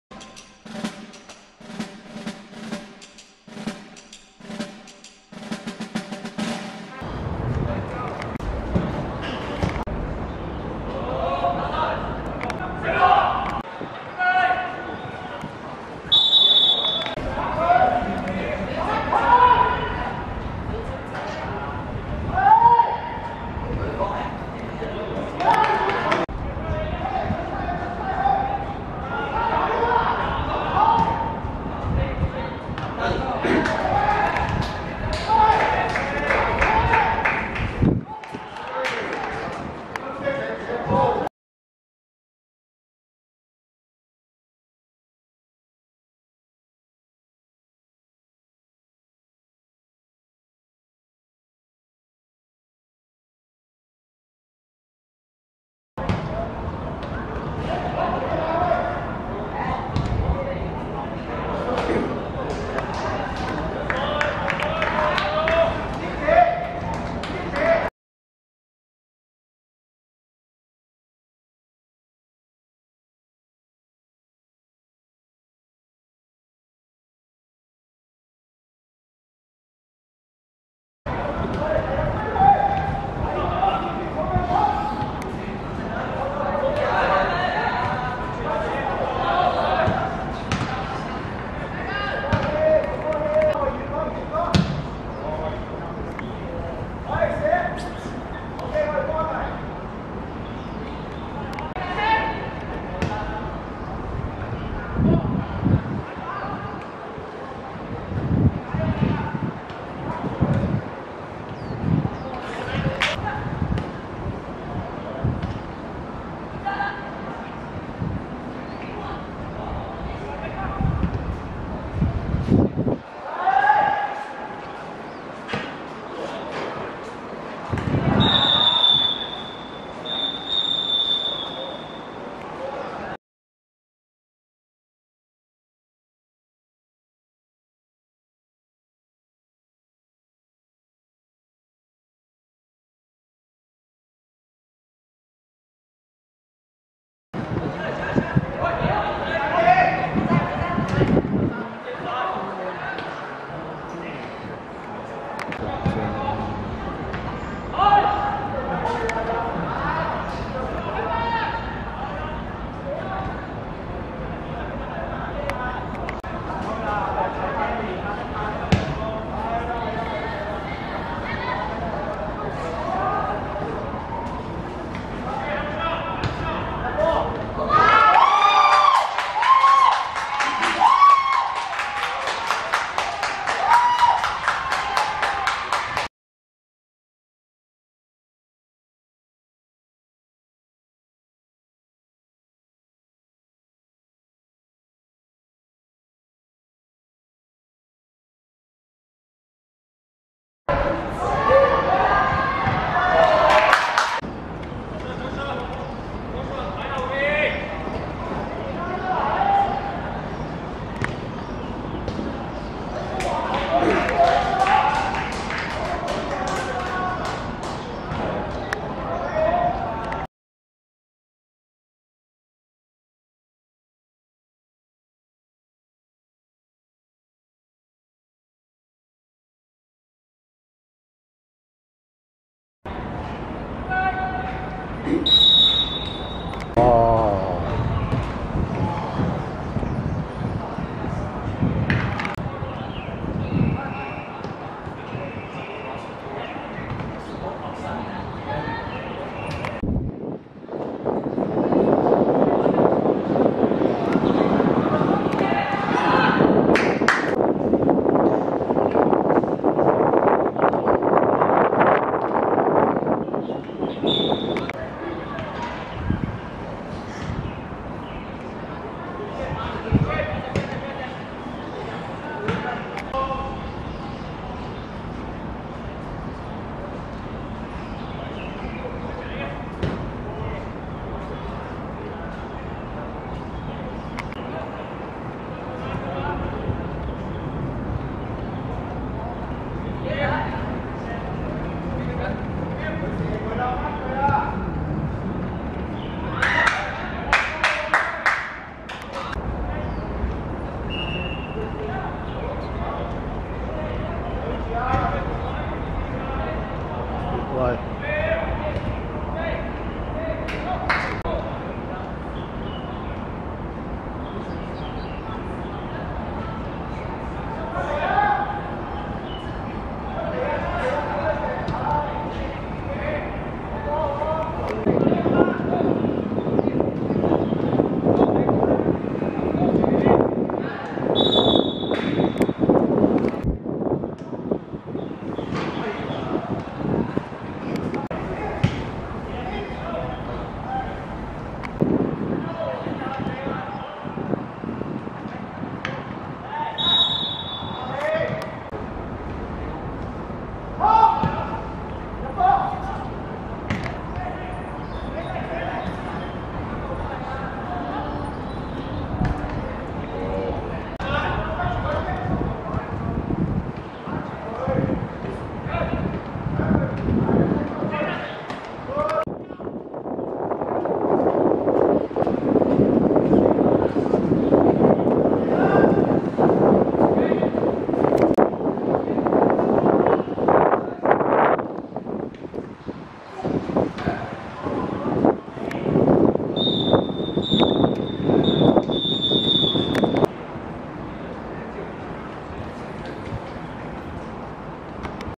拔萃vs聖保羅男女(2018.10.31.D1學界足球乙組)精華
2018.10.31.蒲崗村仿真草塲 15:00 拔萃(藍) 5 : 1 聖保羅男女(白)